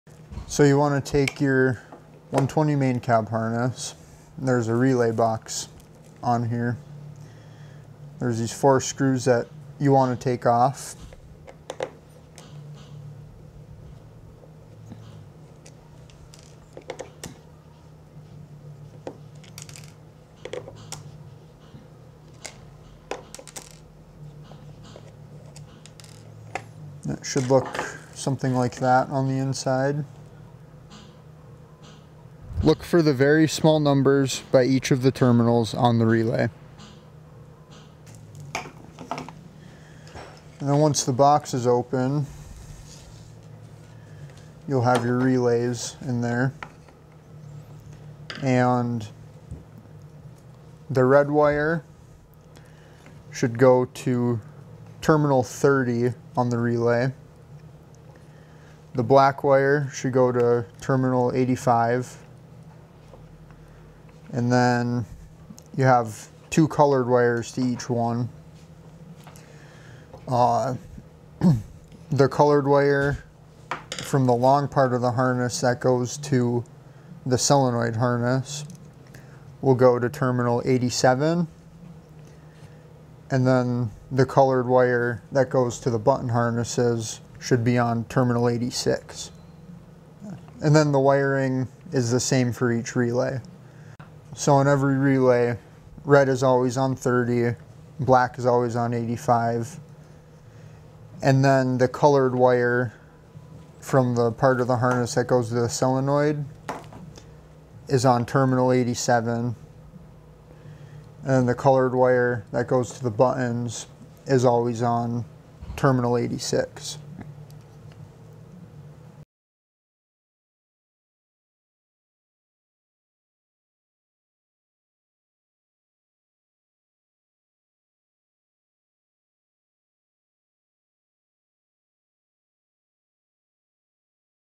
HWP-120 Relay Wiring
If your HWP-120 firewood processor relay box does not have a number 2 on the back, here's how to correct the wiring!